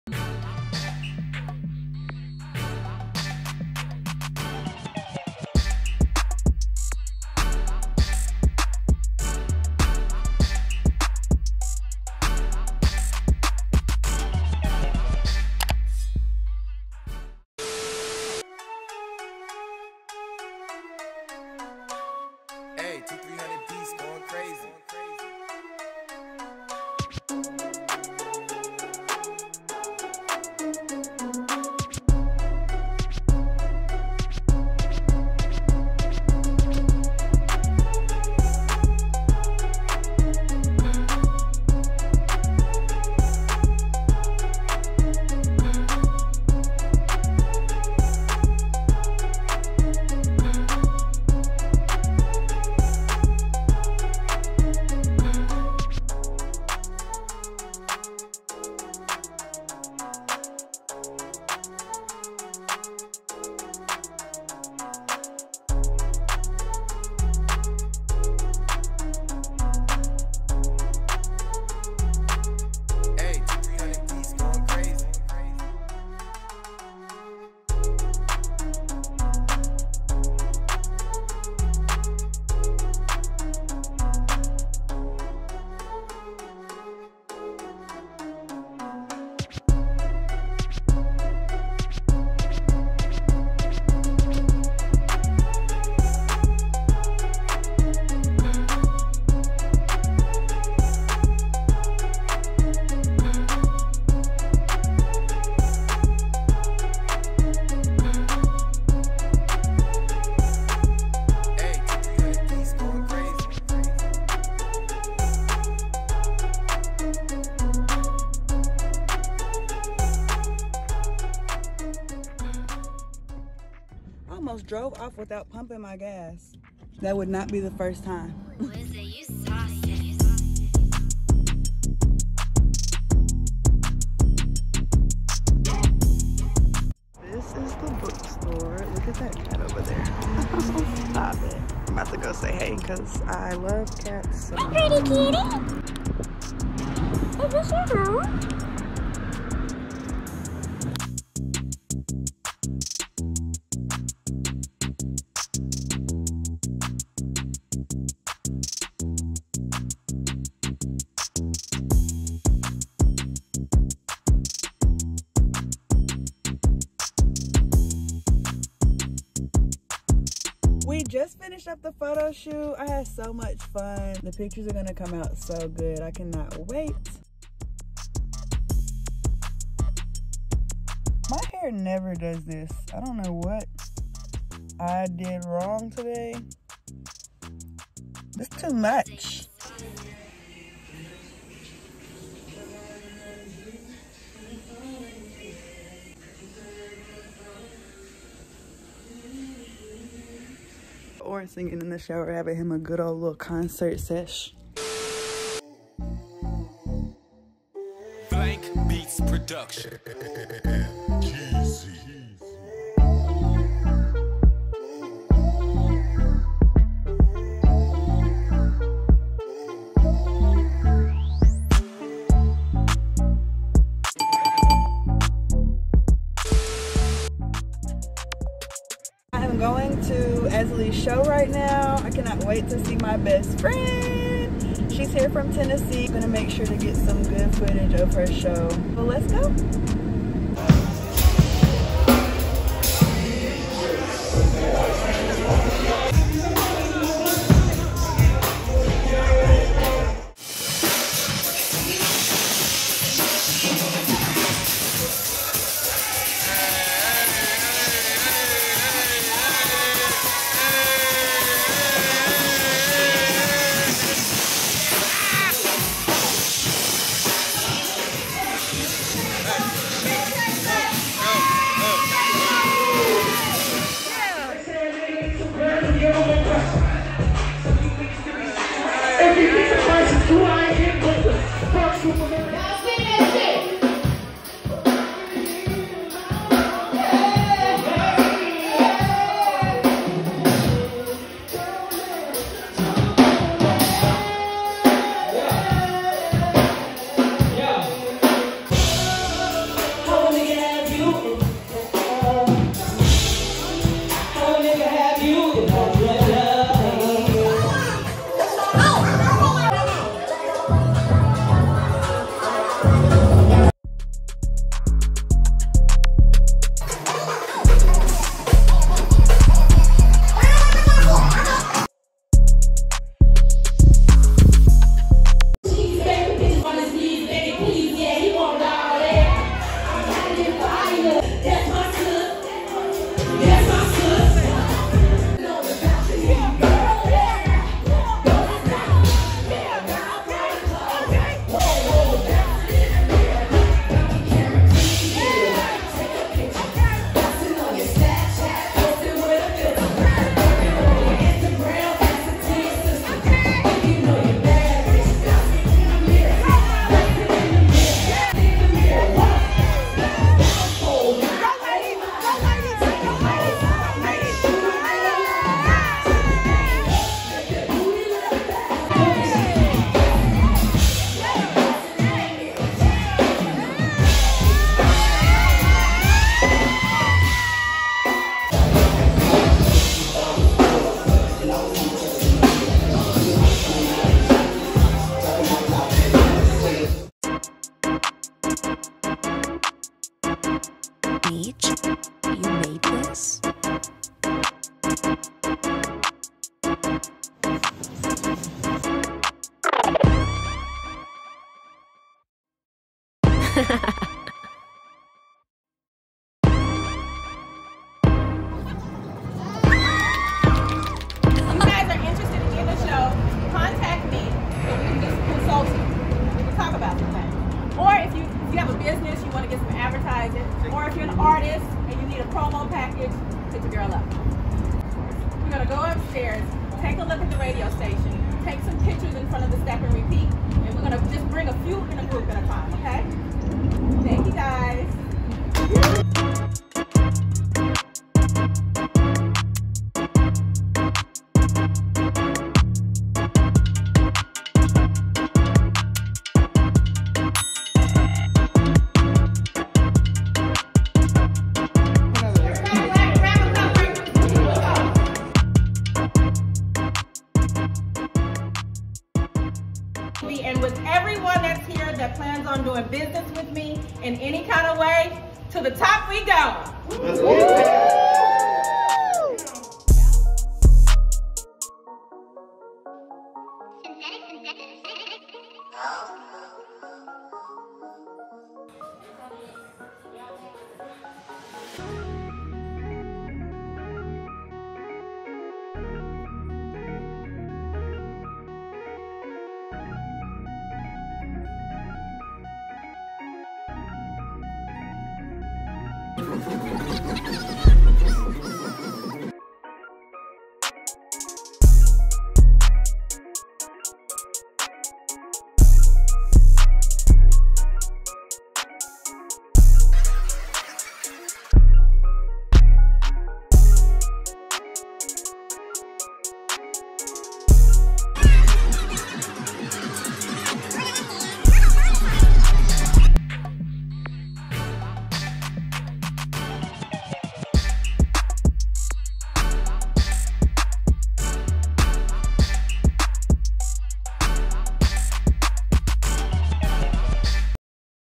Weekly Vlog: GRWM, Brunch in Downtown ATL, Library Photoshoot, Ribbon Cutting, & more.
A week in my life (:

*LASHES: J'Haley Beauty LLC

Time Stamps:
GRWM / Makeup- 0:20
Photoshoot- 02:30
Dinner- 03:25 (Monterrey Mexican Restaurant)
Aszelee J Show- 03:50 (Haven Restaurant & Lounge)
DJChicChick Ribbon Cutting- 06:35
Sunday Brunch- 08:10 (Loca Luna)